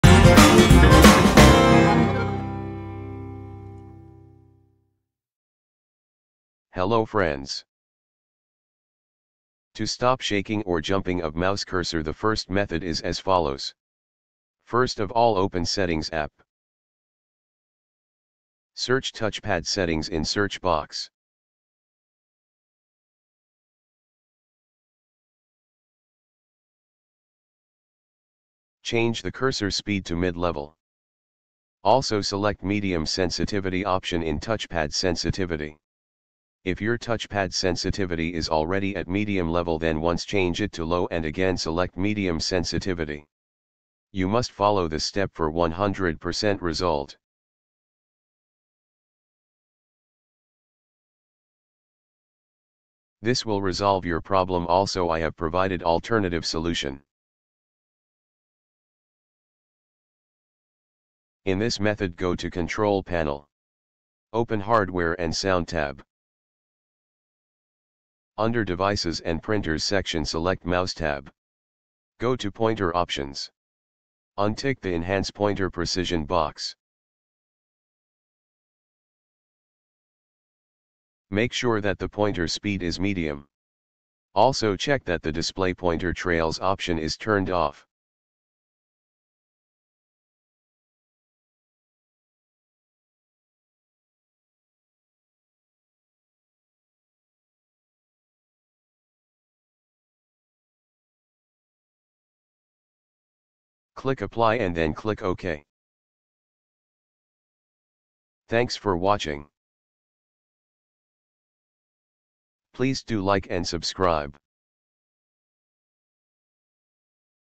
SHAKING/JUMPING OF MOUSE CURSOR PROBLEM SOLVING METHODS IN WINDOWS 10
I will show you "How to stop shaking/jumping of mouse cursor" or any such problem.
There are two methods for solution.